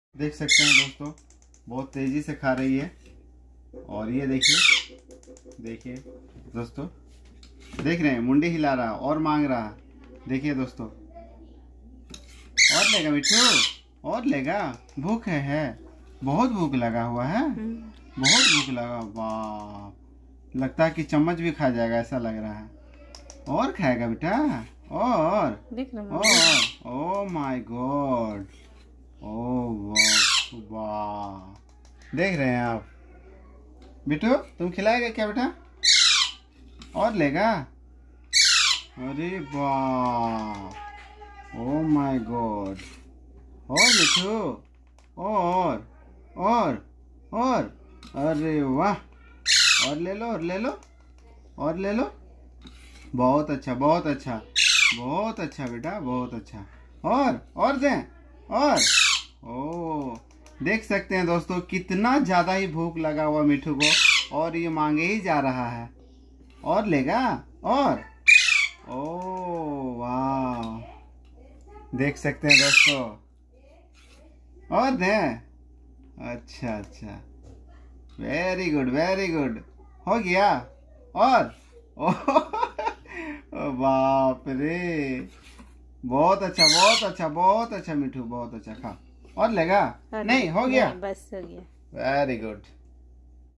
The parrot is hungry and how fast is it eating Sattu
दोस्तों इस वीडियो में आप देखेंगे कि किस तरह से तोता सत्तू को कितना तेज़ी से खा रहा है और कितना ज्यादा ही तोता को भूख लगा हुआ है तो मैं आप सभी को इस वीडियो में दिख रहा हूं तोता किस तरह से कितना तेज़ी से सत्तू को खा रहा है